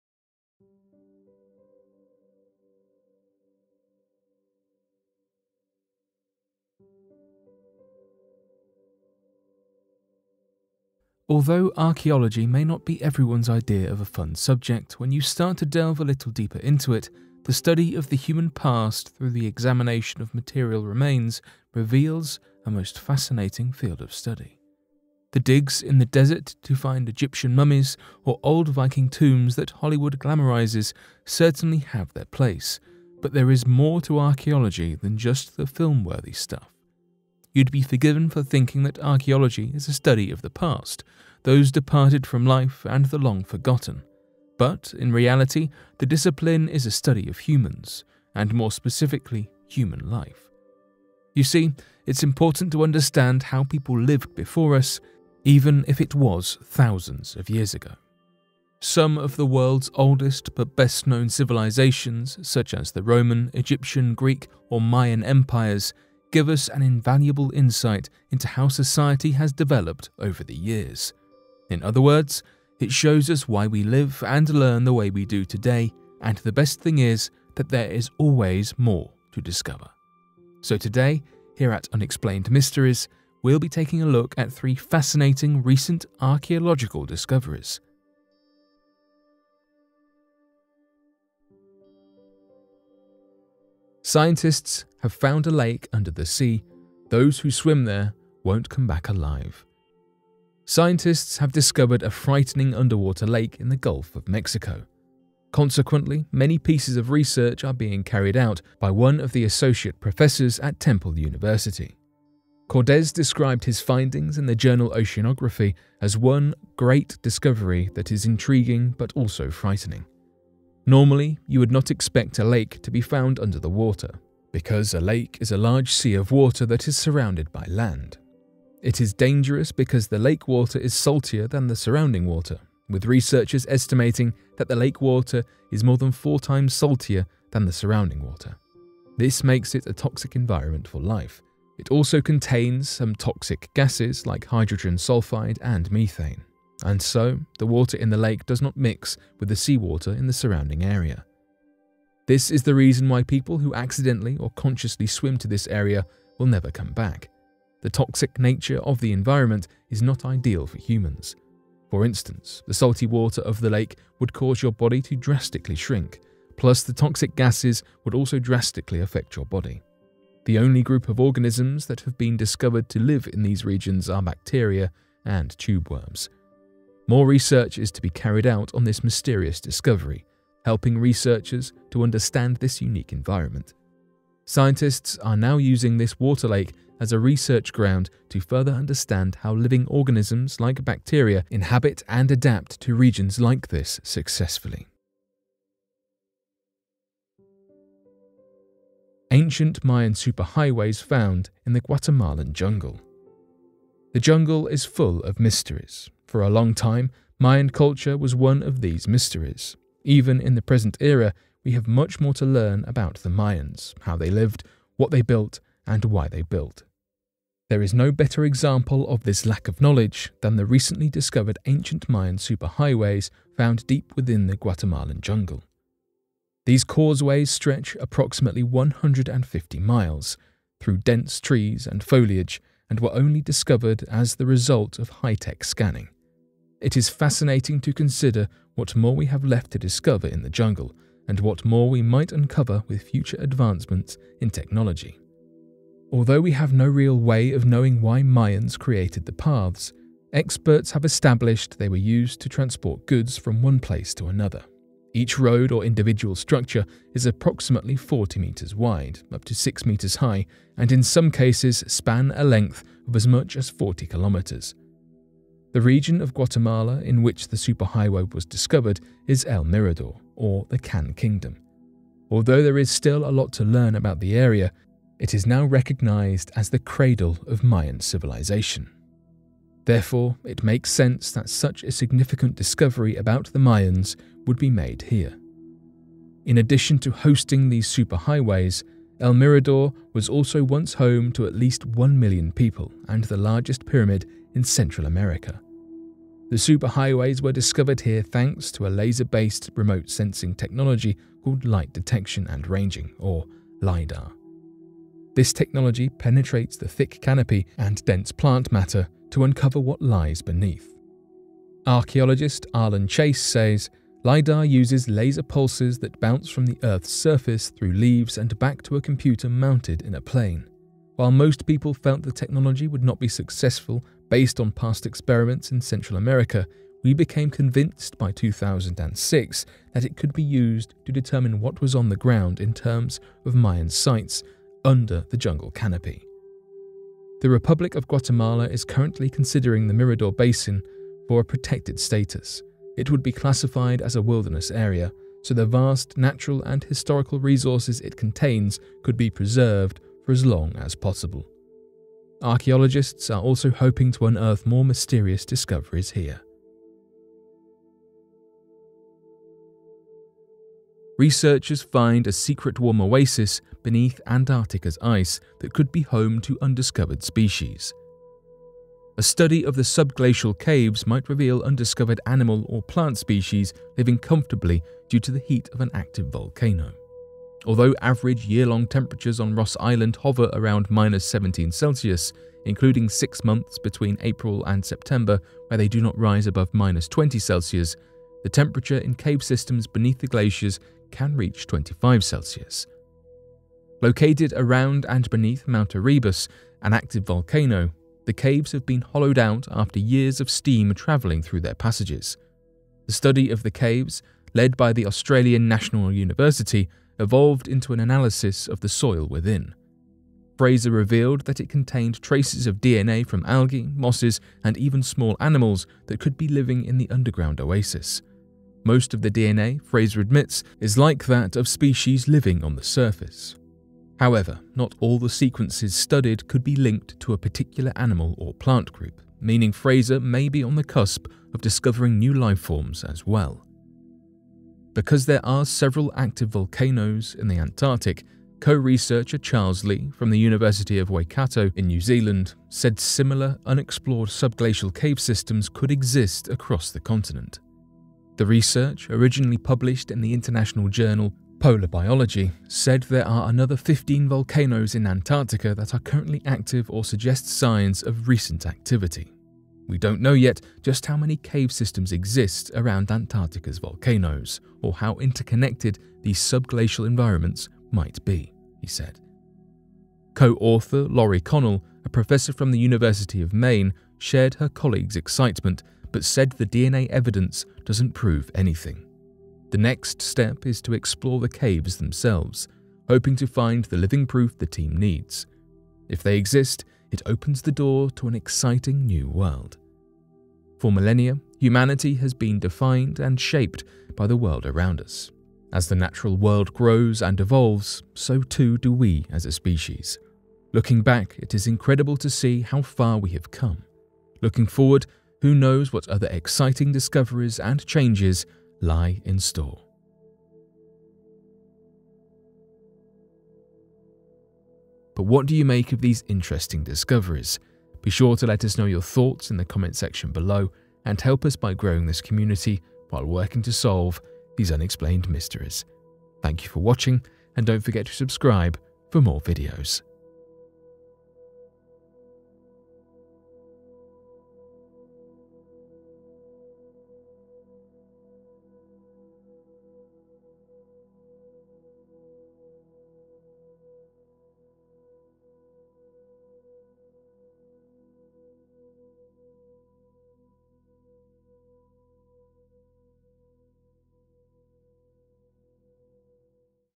Scientists Discover This Chilling Lake Under The Ocean That Destroys Everything It Touches
Scientists discover this chilling lake under the ocean that destroys everything it touches. Today, we take a look at this underwater lake that destroys everything it touches.

Although archaeology may not be everyone’s idea of a fun subject, when you start to delve a little deeper into it, the study of the human past through the examination of material remains reveals a most fascinating field of study. The digs in the desert to find Egyptian mummies or old Viking tombs that Hollywood glamorises certainly have their place, but there is more to archaeology than just the film-worthy stuff.

You’d be forgiven for thinking that archaeology is a study of the past, those departed from life and the long forgotten.